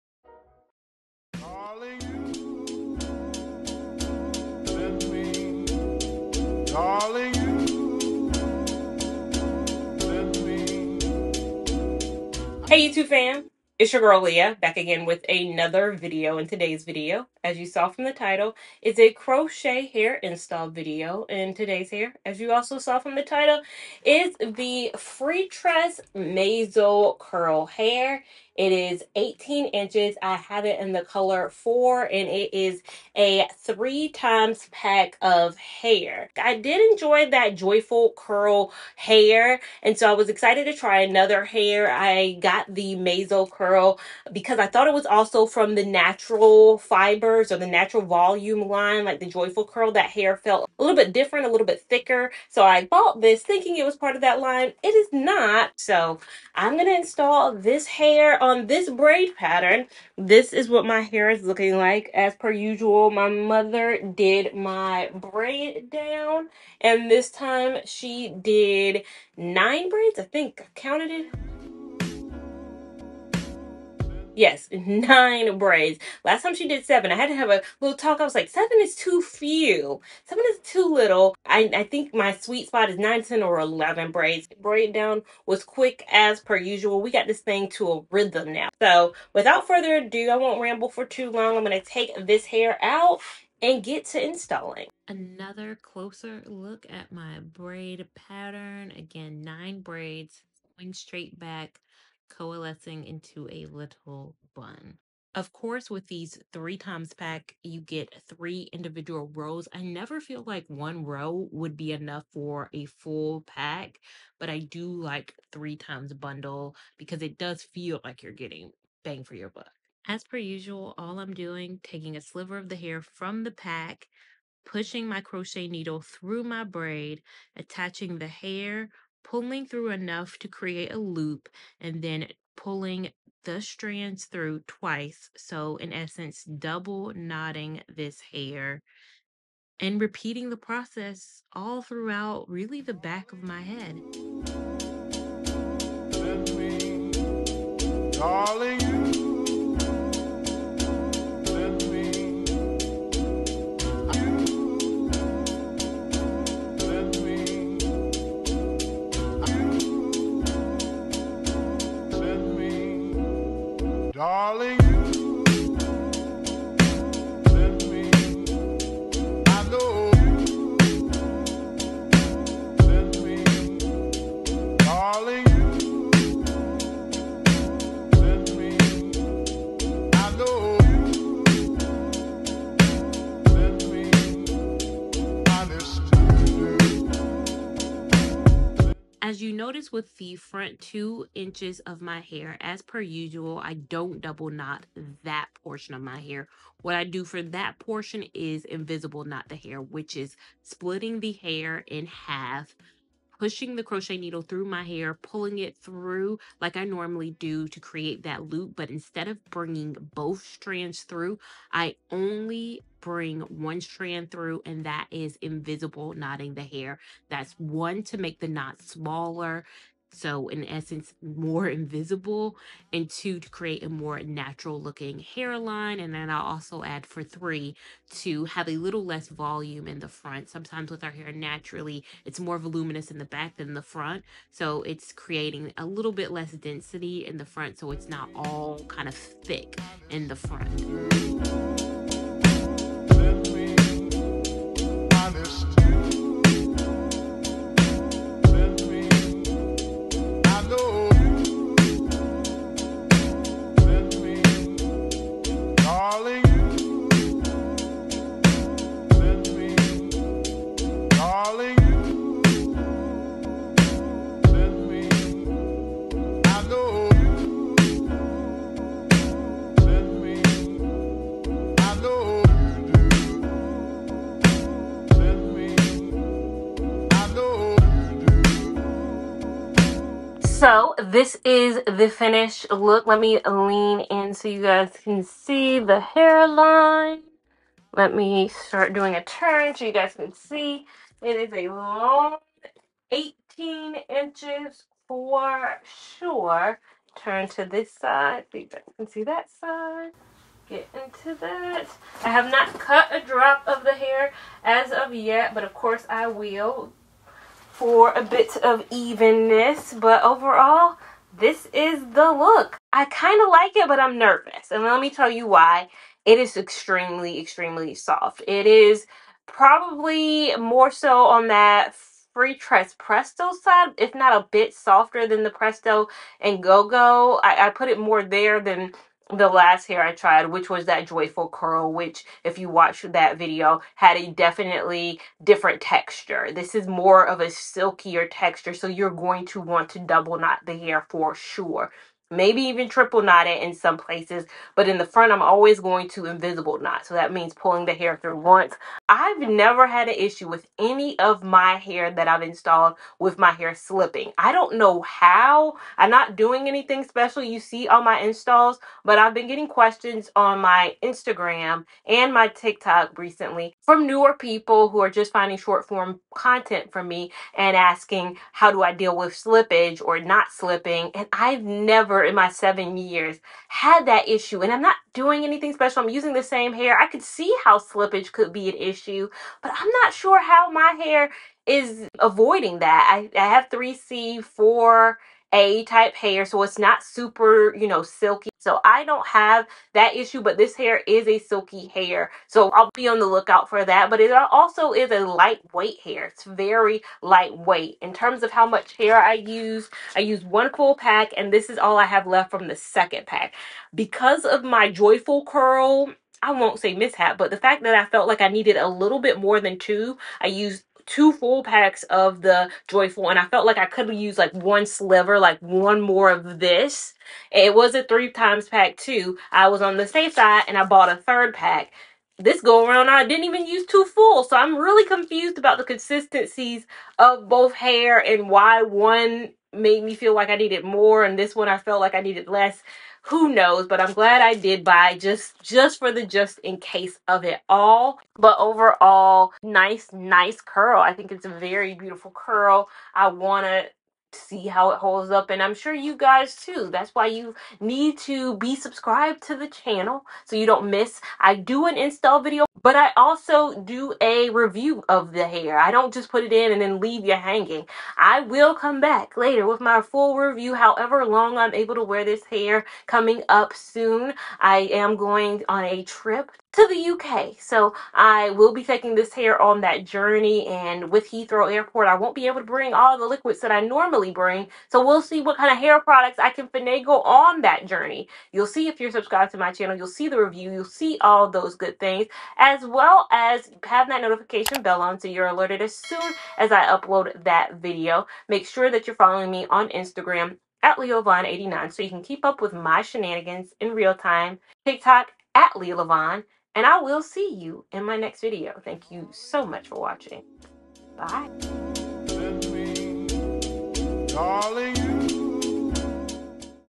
FREETRESS MAZO CURL CROCHET HAIR INSTALL| LIA LAVON
⭐️Hair Information⭐️
Freetress Mazo Curl Crochet Hair
Length - 18 inches
Color - 4
Packs Used - 2

Hawaiian Silky Miracle Worker
Amazon Storefront ⭐️